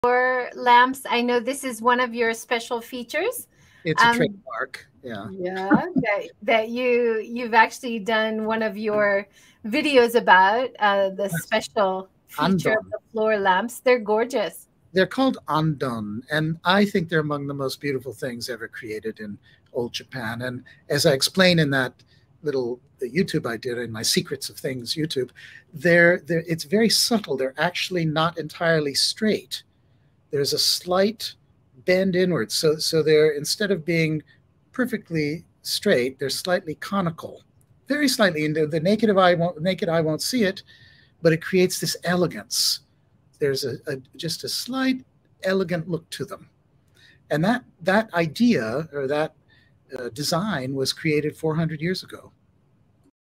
Elegant Japanese ANDON floor lamps perfect in traditional homes with dark wood - Alex Kerr
Japanologist Alex Kerr talks of his love of the traditional Japanese ANDON lamps that he has specially made for the old house renovation projects he does around Japan.